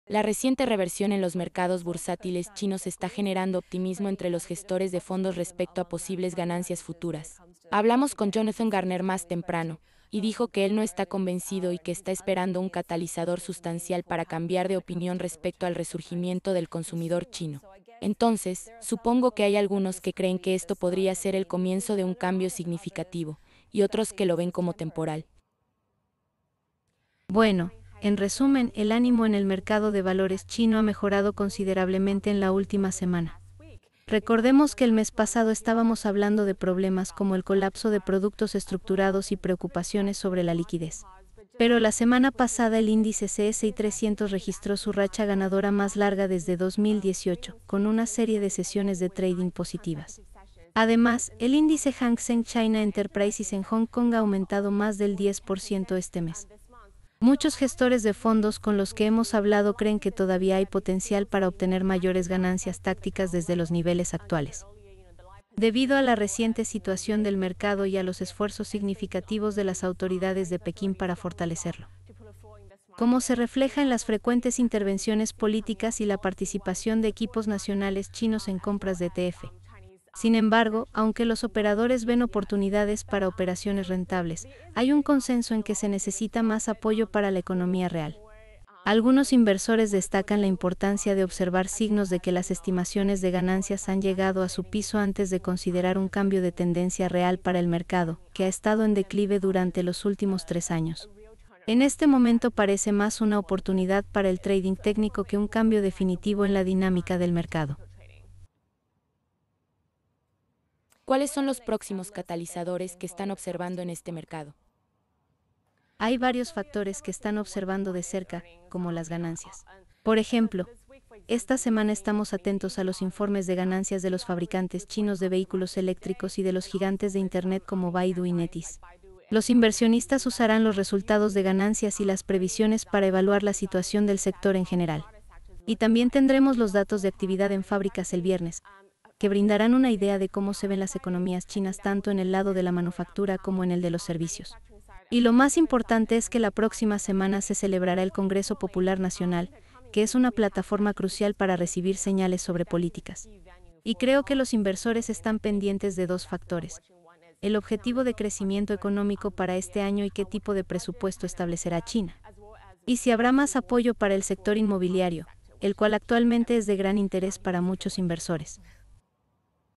Lo último en acciones de China: el rebote estelar hace que los operadores apuesten por más ganancias
Un notable repunte en las acciones chinas, que han pasado de ser las peores a las mejores del mundo, está generando un optimismo cauteloso sobre la continuidad del alza, siempre y cuando las políticas favorables del mercado se mantengan. Charlotte Yang explica en Bloomberg Television.